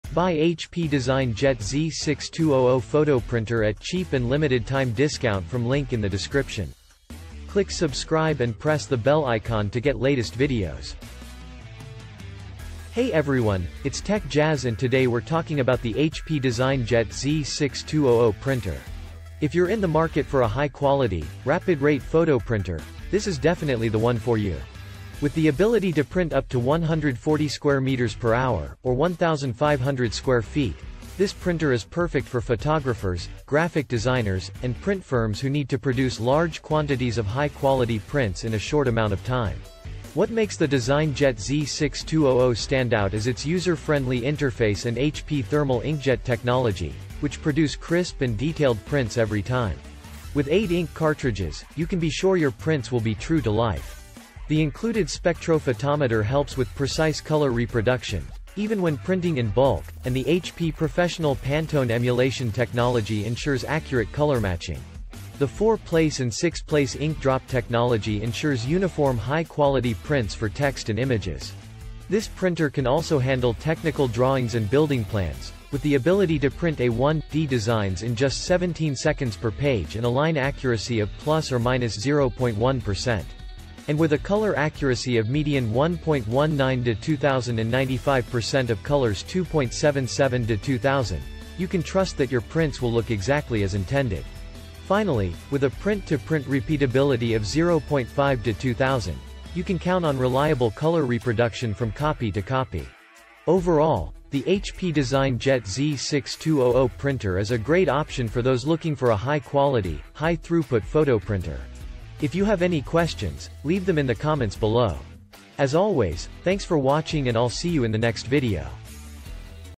HP DesignJet Z6200 Photo Printer Review
Buy HP DesignJet Z6200  Photo Printer at cheap and limited time discount from

The HP DesignJet Z6200 Printer is capable of producing prints of a high quality both rapidly and in large quantities. It has a print speed of up to 140 square meters per hour, making it ideal for large-scale printing.

High-throughput printing is no problem for this printer's intuitive interface, making it a great choice for photographers, graphic designers, and printing companies.

Using HP's Thermal Inkjet technology, the DesignJet Z6200 consistently delivers crisp, high-definition prints. The printer has room for eight different ink colors, including chromatic red, magenta, matte black, picture black, yellow, light cyan, light gray, and light magenta.

The Z6200 printer uses HP Vivid Photo Inks, which were developed particularly for printing photographs, to provide colors that are accurate representations of the originals. Built-in spectrophotometer guarantees accurate color reproduction, even in large print runs.

Thanks to HP Professional PANTONE Emulation, you can count on perfect color consistency and matching every time. Quickly and easily print technical drawings, construction plans, and other comparable documents with the A1/D design capability in approximately 17 seconds per page.